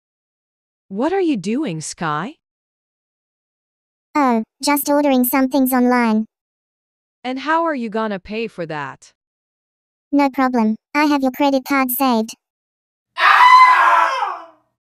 Dog Orders Online🐶Cute Maltese Dog - Funny Shorts
Do you have the same problem? Does your dog order things online and pays with your credit card? Watch new cute adventures of this funny Maltese dog.